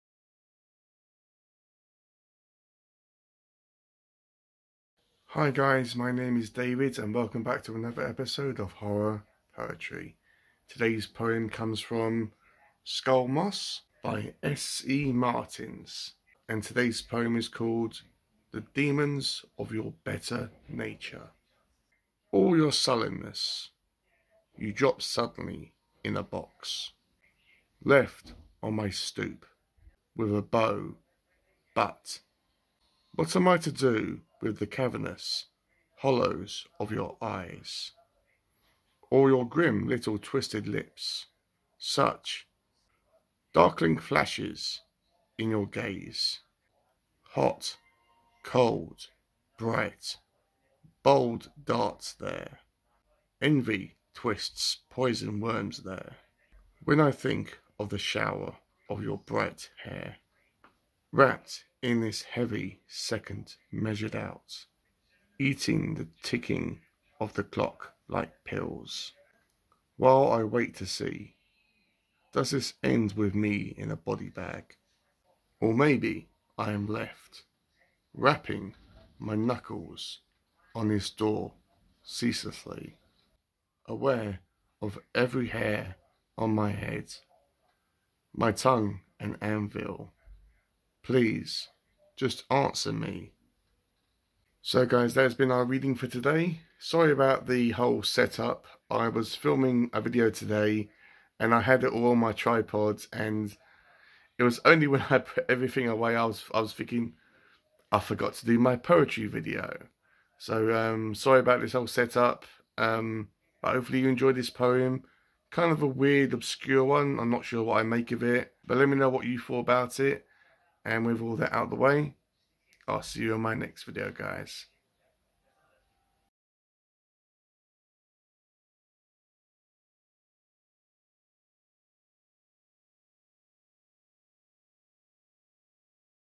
Horror Poetry - Episode 49💀The Demons Of Your Better Nature by S.E. Martens💚
During today's video, I read a poem from the collection Skull Moss by S.E. Martens, called The Demons Of The Better Nature. Enjoy!